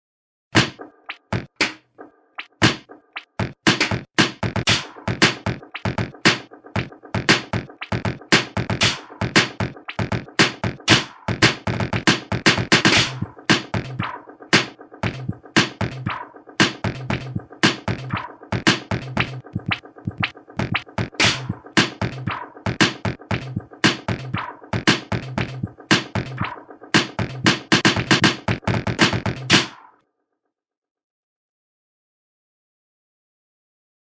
A Beat Made with This Thing
Pretty sure I've invented the new dubstep. Sounds made with a piece of aluminum flashing put together in garage band.